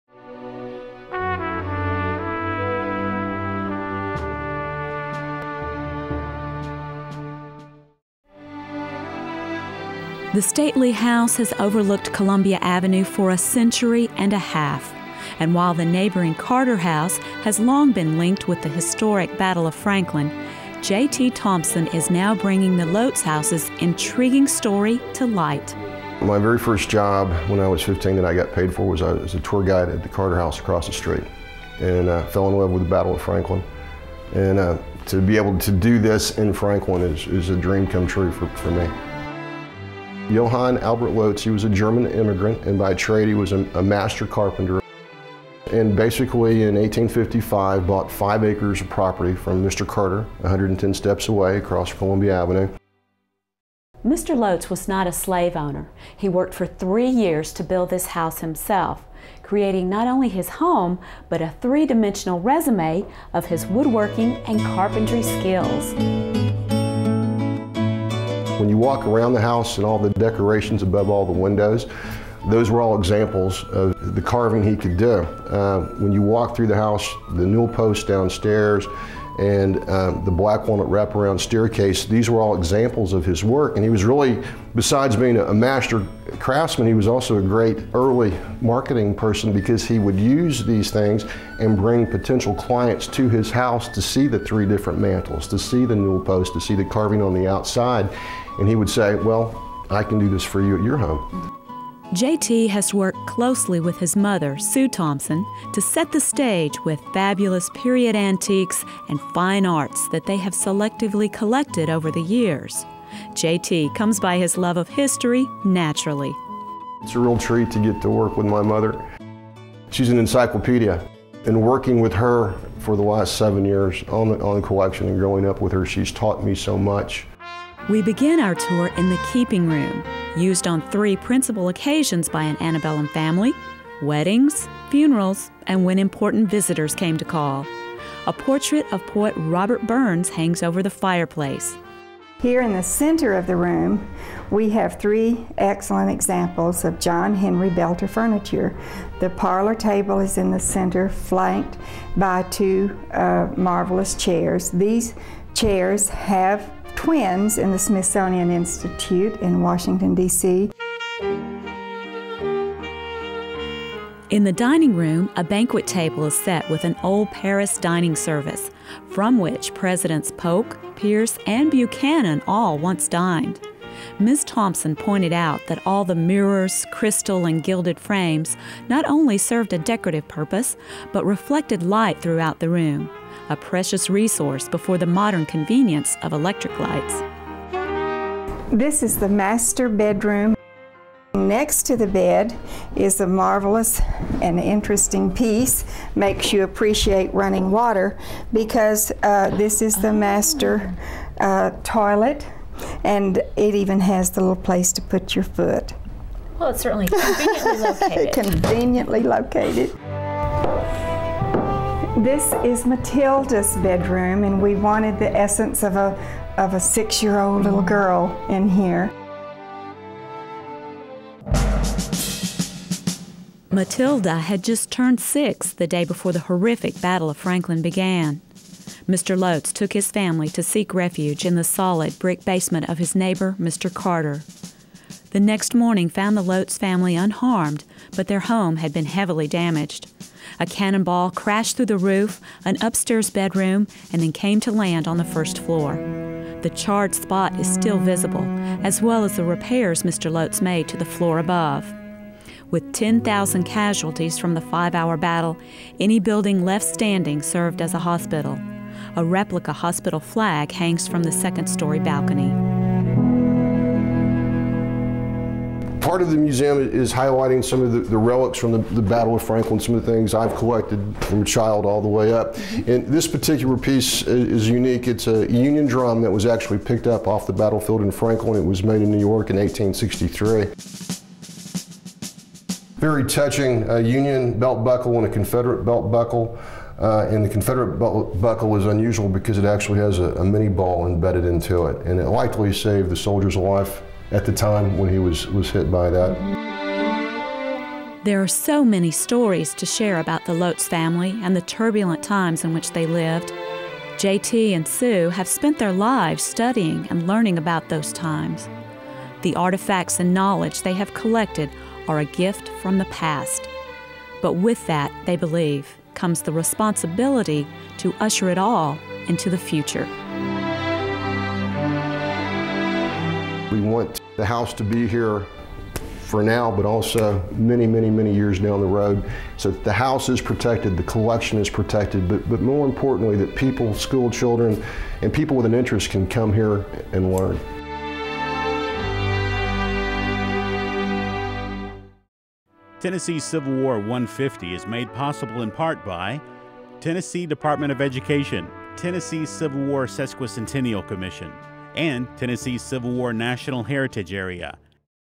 Lotz House
About 20,000 Federal troops established their main line 100 yards south of the Lotz house. After the battle, the ground between the Carter and Lotz houses was so filled with bodies that they couldn't take a step without walking on one of them. Learn more about the Lotz House and its significance during the Civil War.